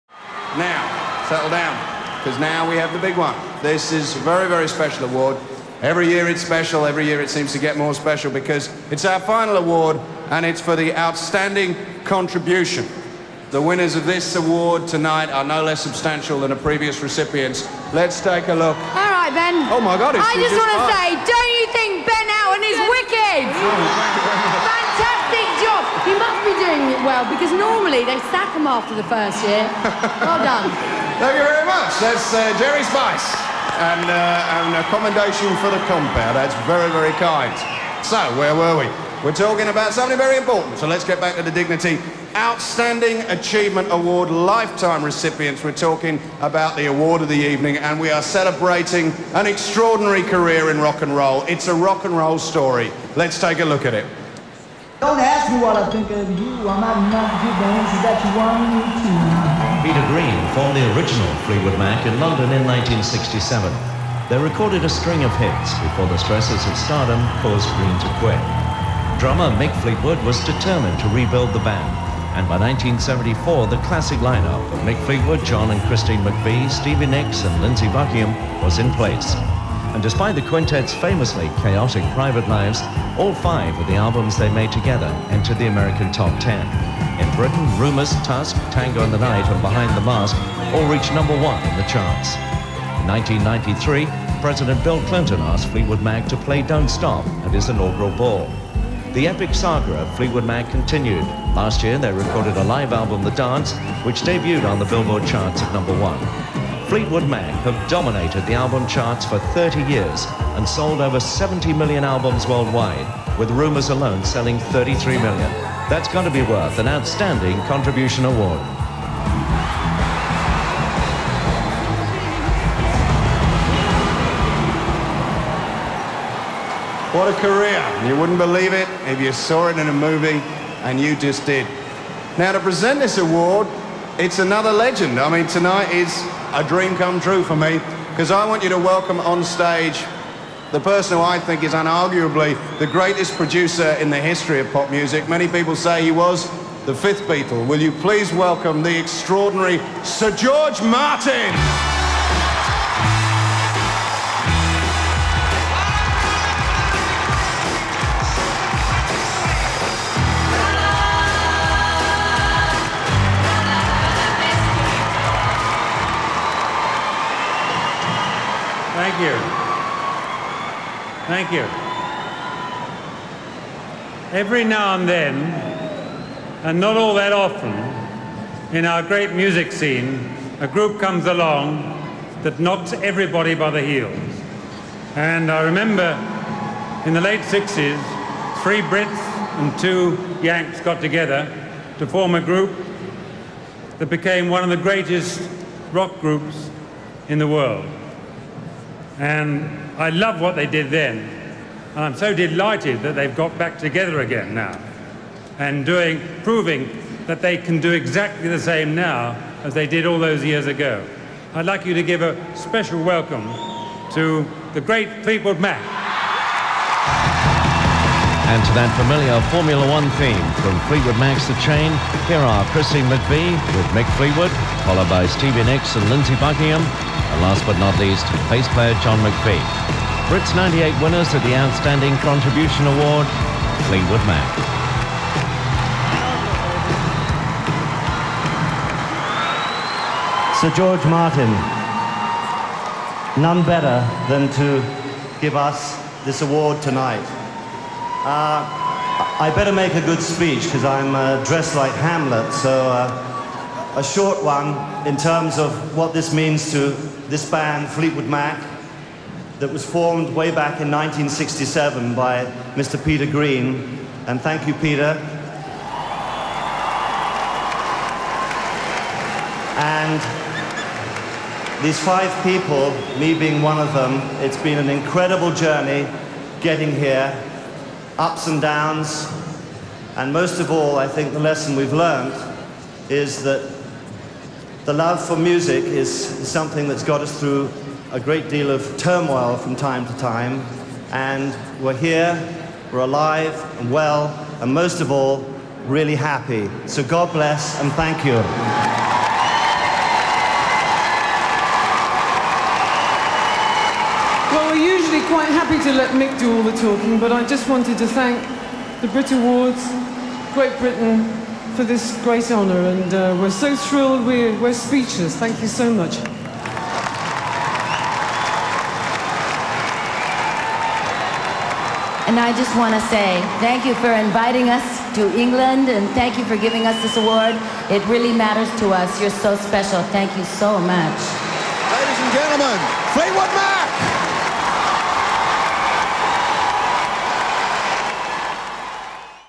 Fleetwood Mac win Outstanding Contribution presented by Sir George Martin | BRIT Awards 1998
Sir George Martin presented the BRIT Award for Outstanding Contribution to Music at The BRIT Awards 1998 to Fleetwood Mac